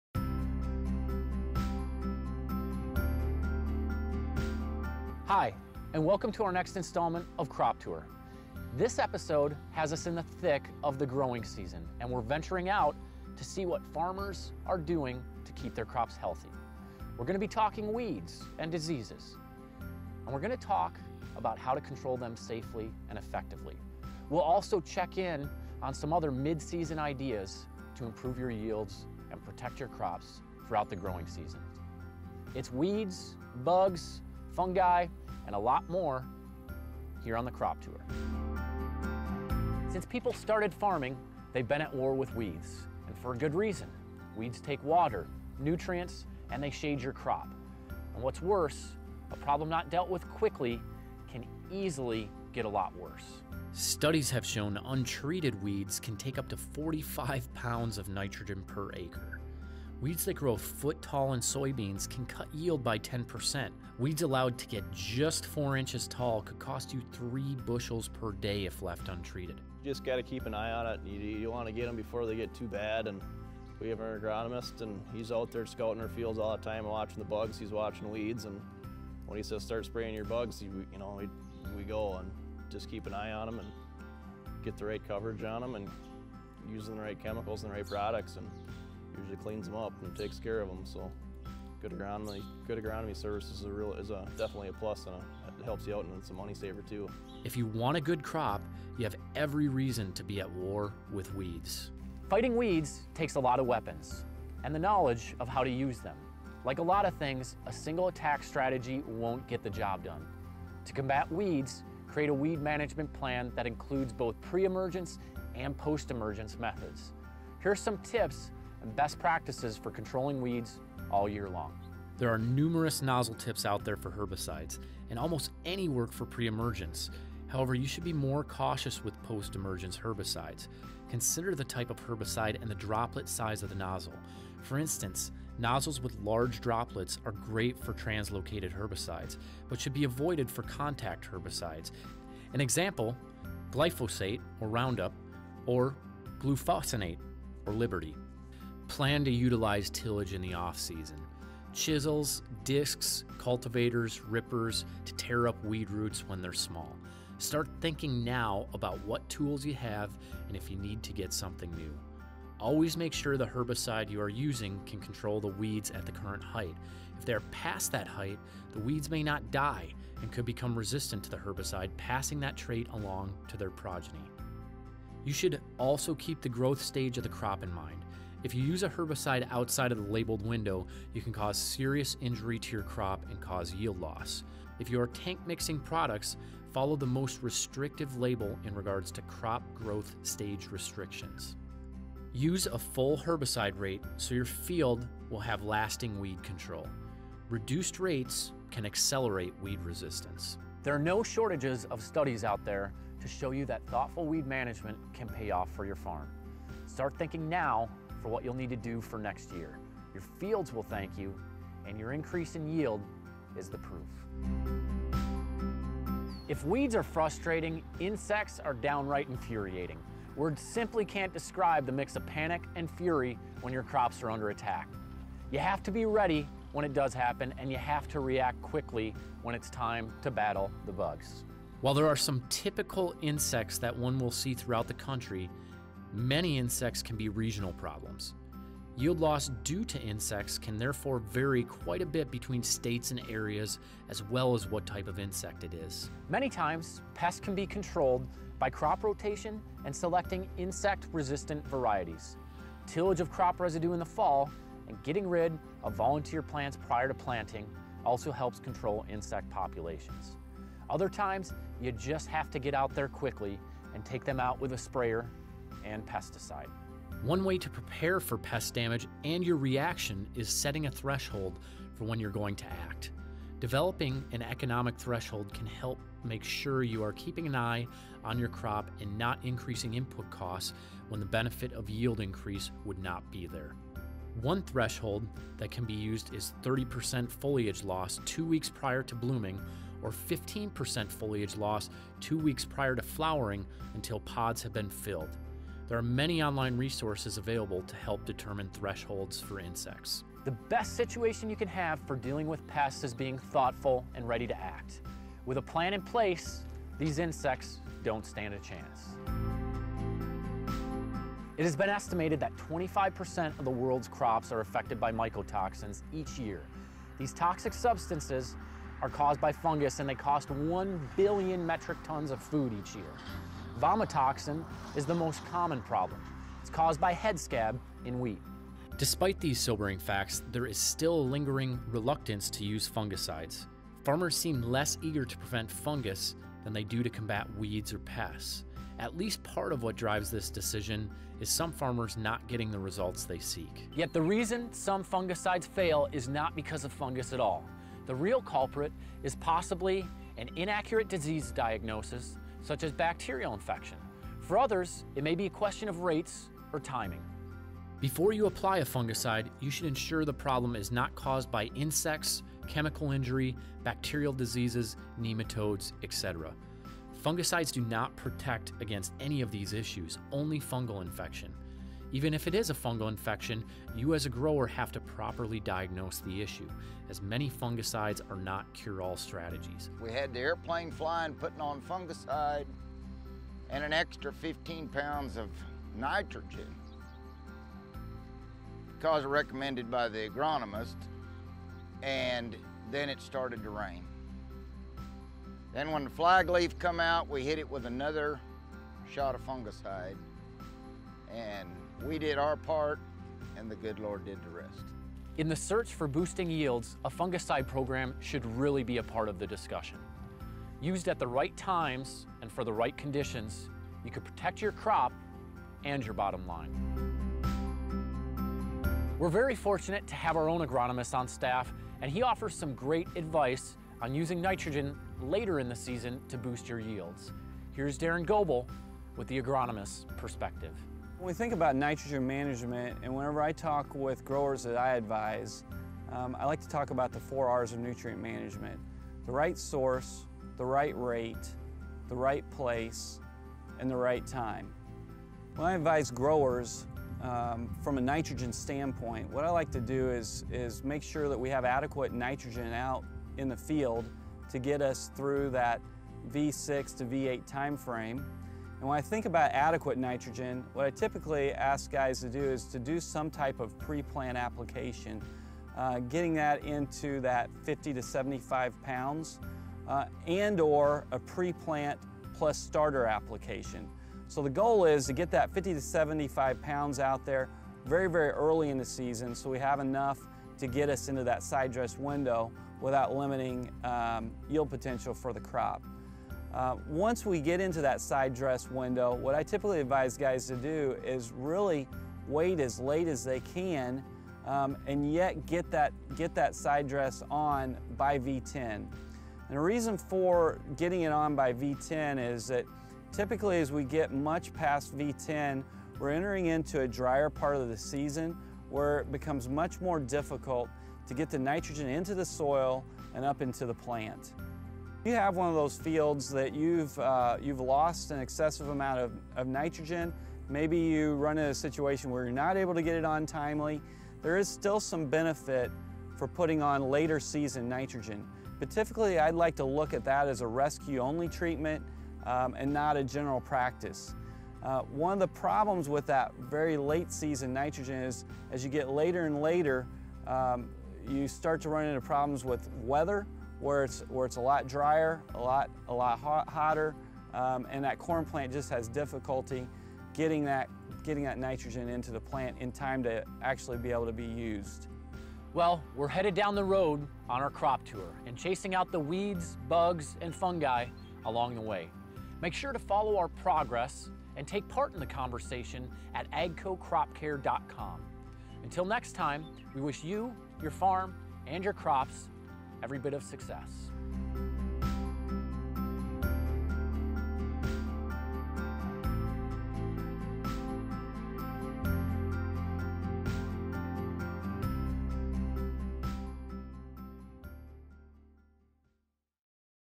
Crop Care Episode Two: Spraying
Episode two of Crop Tour has us in the thick of the growing season. We’ll be visiting with farmers to see what they’re doing to keep their crops healthy. We’ll also be sharing best practices around how to control weeds and diseases safely and effectively.